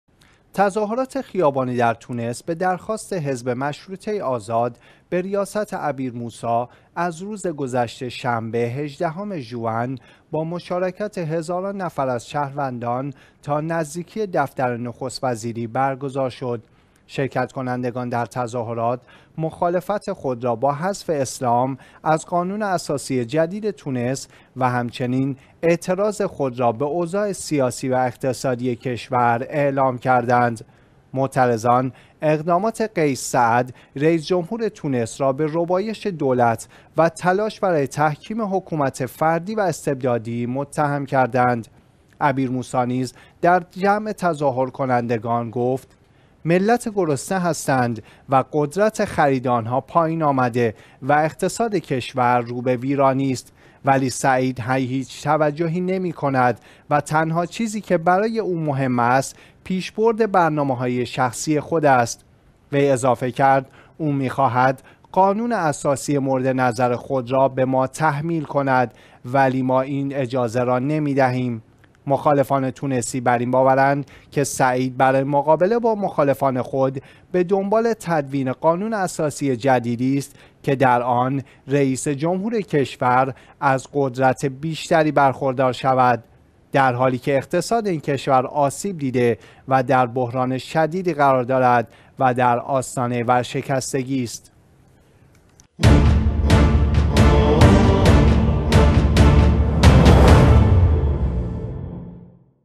تظاهرات شهروندان تونسی‌ در مخالفت با قانون اساسی جدید و حذف اسلام از قانون اساسی
تظاهرات خیابانی در تونس به درخواست حزب مشروطه آزاد به ریاست عبیر موسی از روز گذشته ، شنبه ، ۱۸ ژوئن ، با مشارکت هزاران نفر از شهروندان تا نزدیکی دفتر نخست وزیری برگزار شد.

شرکت کنندگان در تظاهرات ، مخالفت خود را با حذف اسلام از قانون اساسی جدید تونس و همچنین اعتراض خود را به اوضاع سیاسی و اقتصادی کشور اعلام کردند.

معترضان اقدامات قیس سعید رئیس‌جمهور تونس را به ربایش دولت و تلاش برای تحکیم حکومت فردی و استبدادی متهم کردند.

عبیر موسی نیز در جمع تظاهر کنندگان گفت: ملت گرسنه هستند و قدرت خرید آنها پایین آمده و اقتصاد کشور روبه ویرانی است ولی سعید هیچ توجهی نمی‌کند و تنها چیزی که برای او مهم است پیشبرد برنامه‌های شخصی خود است.
وی اضافه کرد: او می‌خواهد قانون اساسی مورد نظر خود را به ما تحمیل کند ولی ما این اجازه را نمی‌دهیم.

مخالفان تونسی بر این باورند که سعید برای مقابله با مخالفان خود ، به دنبال تدوین قانون اساسی جدیدی است که در آن رئیس‌جمهور کشور از قدرت بیشتری برخوردار شود ، درحالی‌که اقتصاد این کشور آسیب دیده در بحران شدیدی قرار دارد و در آستانه ورشکستگی است.